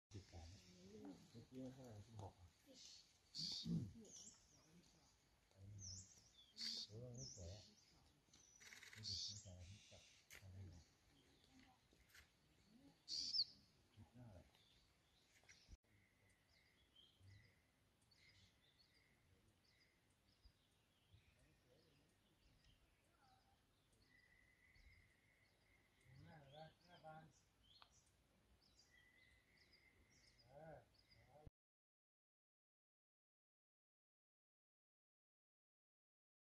นักท่องเที่ยวแห่สัมผัสทุ่งดอกไม้เมืองหนาวชมทะเลหมอก
ผู้สื่อข่าวรายงานว่าว่า บรรยากาศเช้าวันนี้อากาศในพื้นที่ทั่วไปของจังหวัดเชียงใหม่ยังคงเย็นสบายแต่ก็ยังไม่ถึงกับหนาวมากอากาศกำลังดี